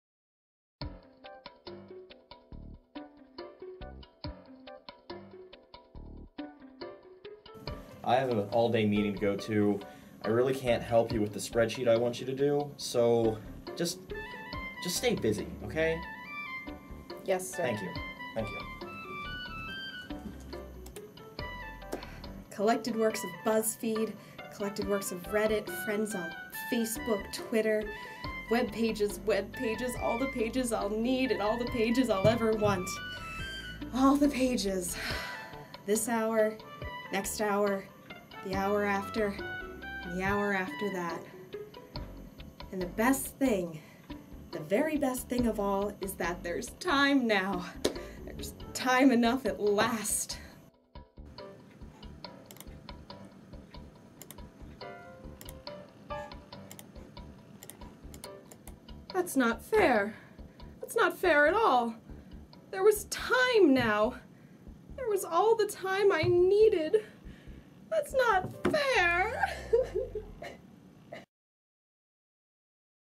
Twilight Zone - All the time in the world
Missy finds she now has all the time in the world to browse the internet... but wait...